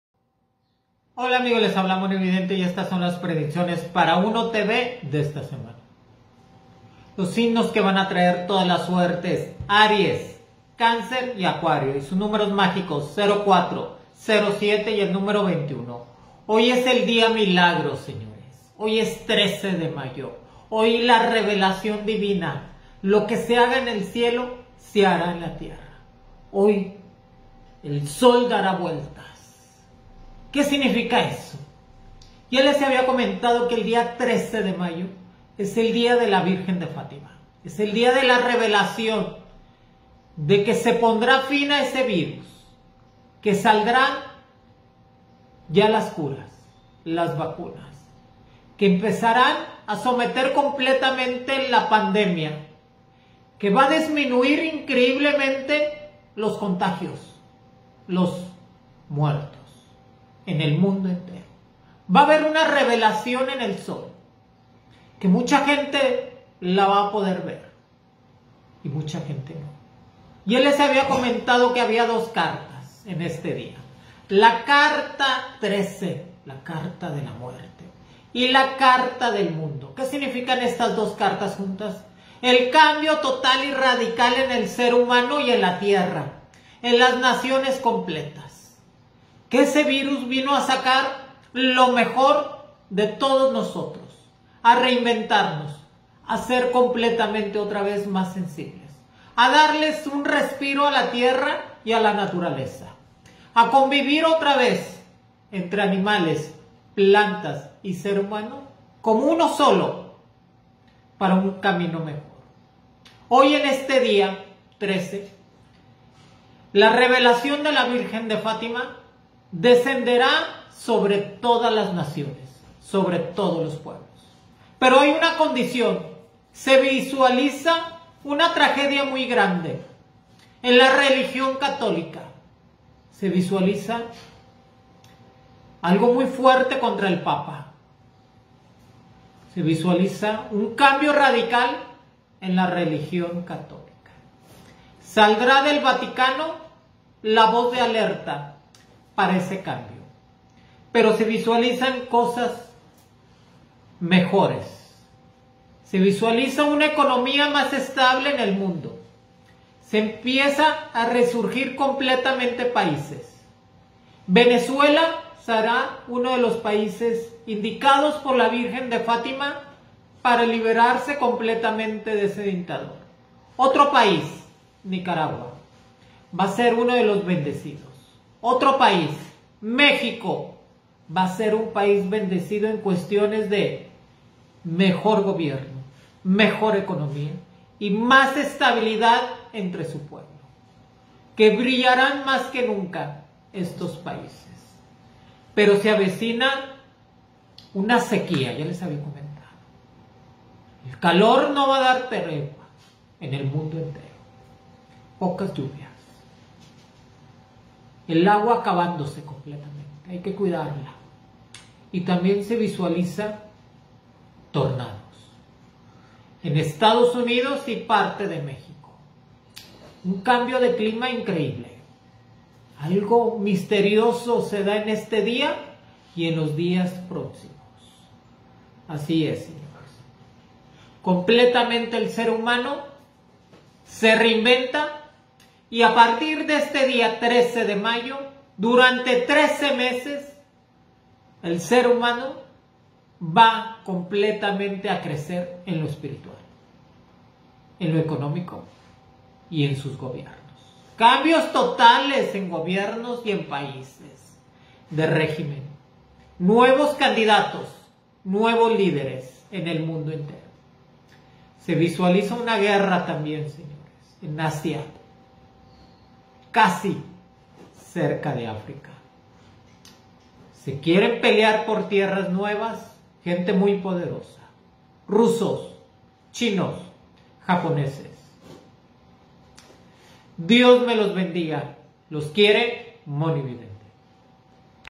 Nuevas Predicciones de Mhonividente
En esta semana Mhonividente nos da los numeros y signos de suerte y nos habla sobre el dia milagro 13 de mayo y su significado,Covid19,tragedia en la religion catolica,economia,Venezuela,Nicaragua,Mexico,Sequia en el mundo,Tornados en Estados Unidos y Mexico.